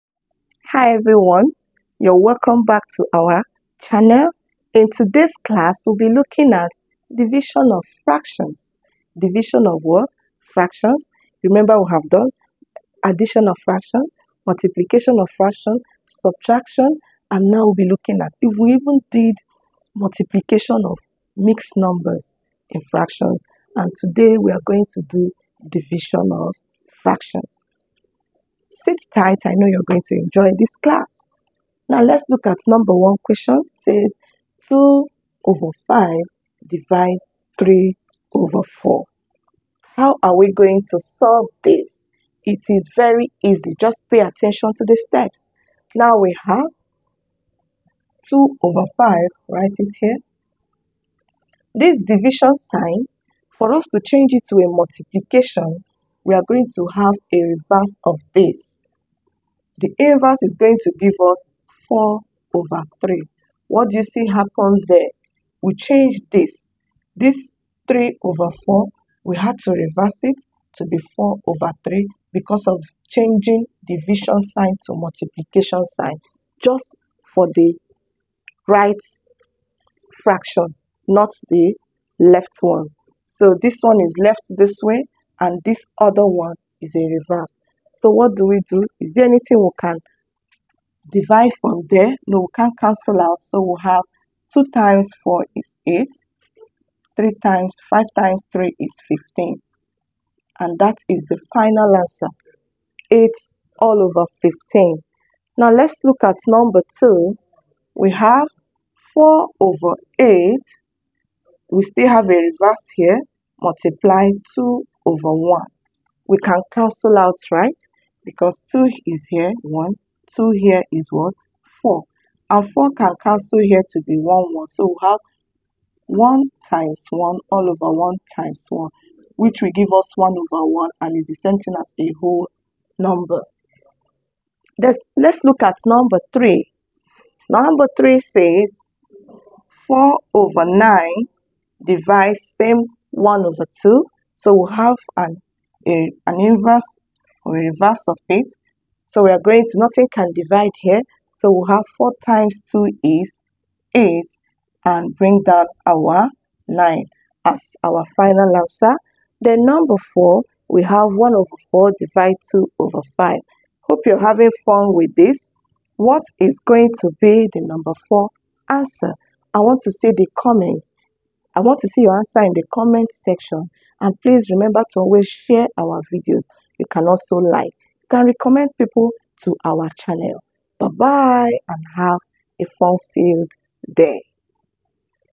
Division of Fractions
Dividing fractions is almost the same as multiplying of fractions.
For the division of fractions, we multiply the first fraction by the reciprocal( inverse)  of the second fraction..
In this video, you will learn more on division of fractions in a fun way.